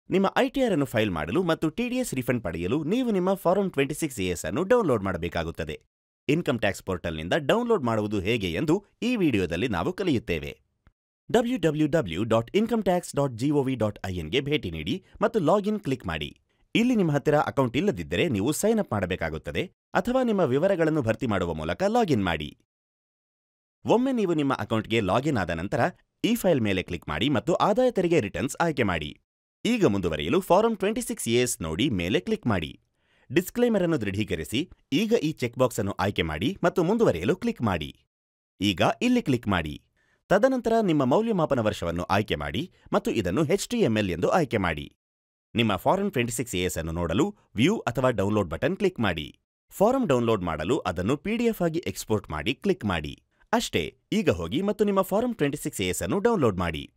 How to Download Form 26AS - Kannada
In this video RummyTime is telling how to download Form 26AS to help Rummy players file their Income Tax Return through Cleartax at Net Zero* Cost.